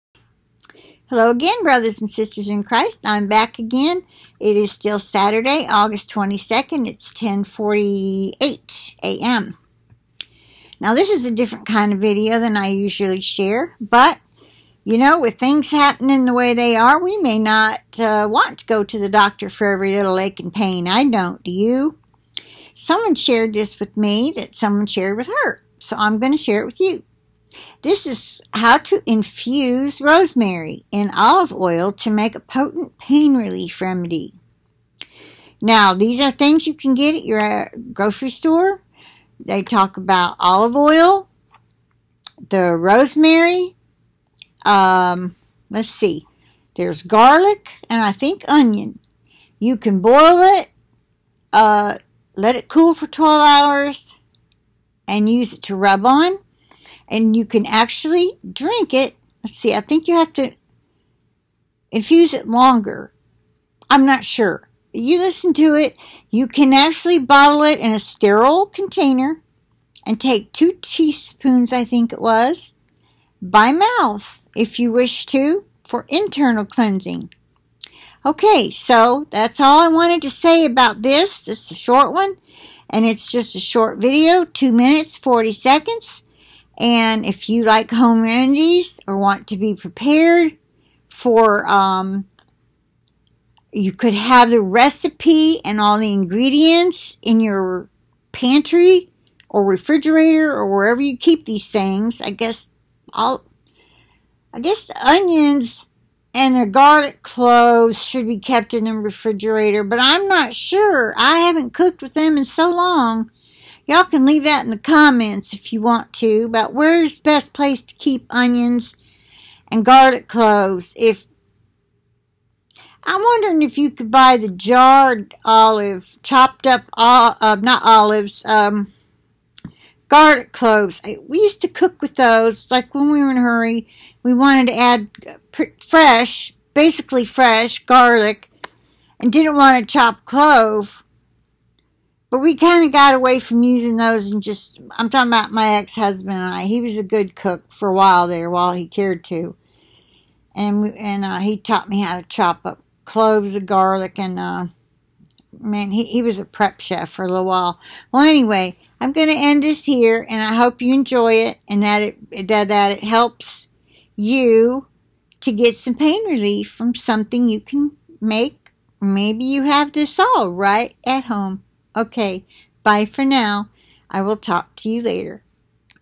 HOW TO MAKE A POTENT PAIN RELIEF REMEDY AT HOME WITH THINGS YOU MAY HAVE IN YOUR KITCHEN
TITLE: "Infuse Rosemary In Olive Oil To Make a Potent Pain Relief Remedy"